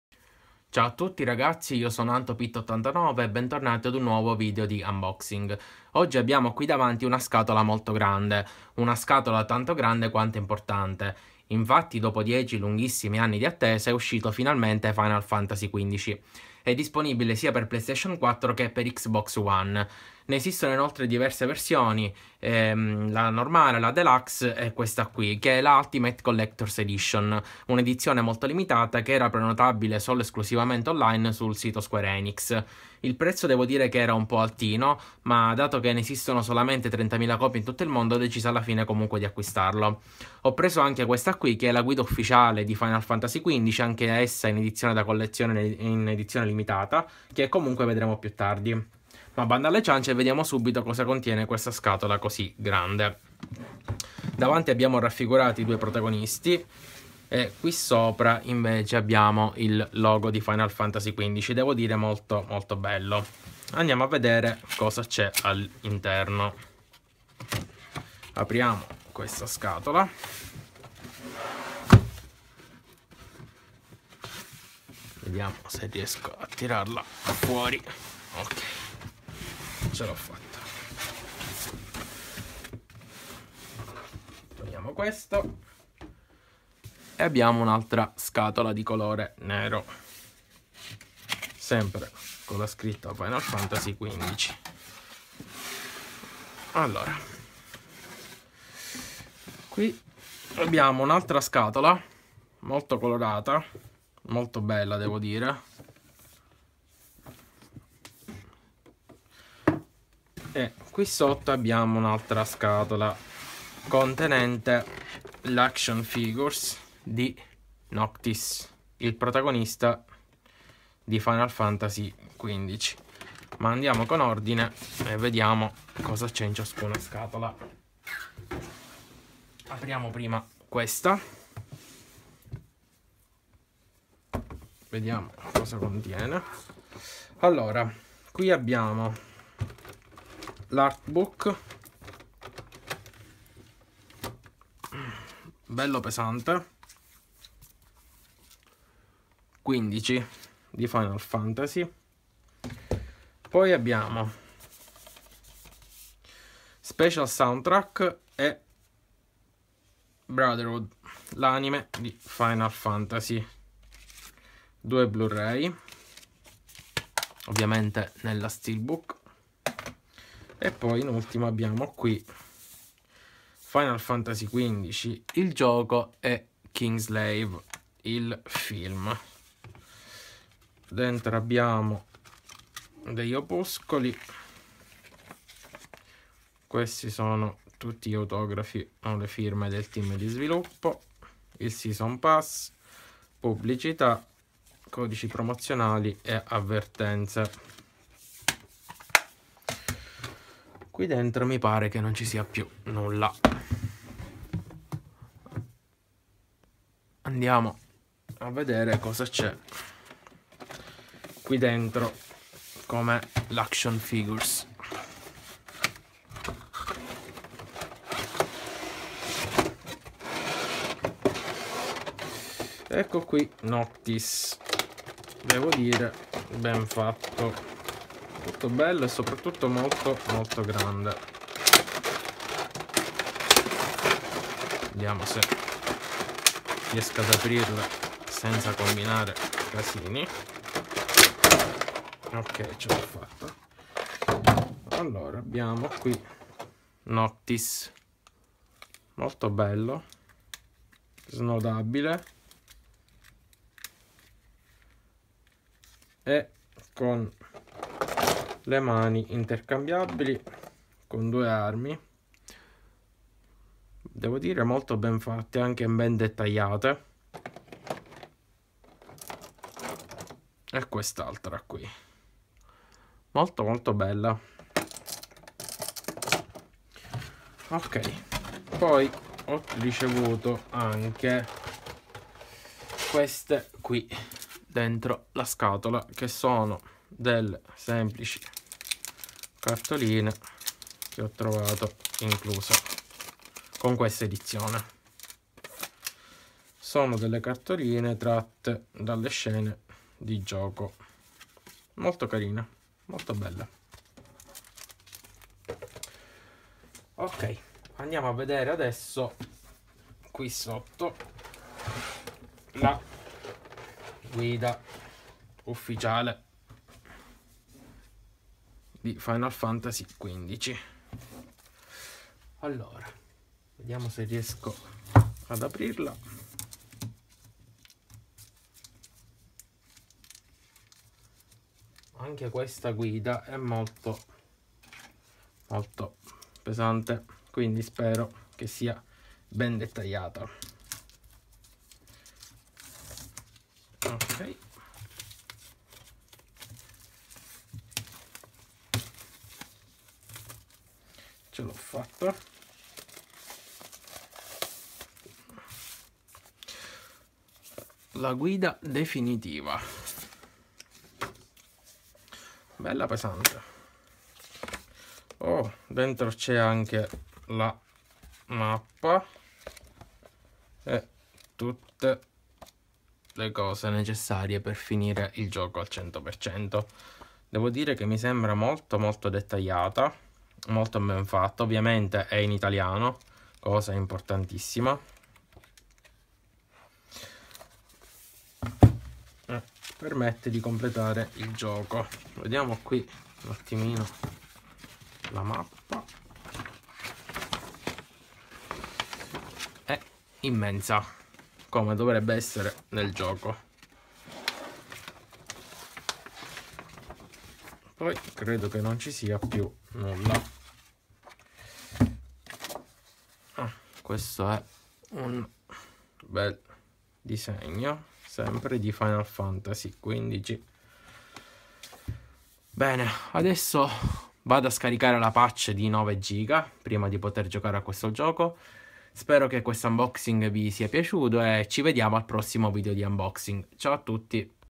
Final Fantasy XV Ultimate Collector's Edition + Guida [Unboxing ITA]
Dopo milioni, anzi miliardi di peripezie e 10 lunghissimi anni di attesa, è uscito Final Fantasy XV. Ecco a voi l'unboxing della Ultimate Collector's Edition disponibile solo in 30000 copie in tutto il mondo! Buona visione 😁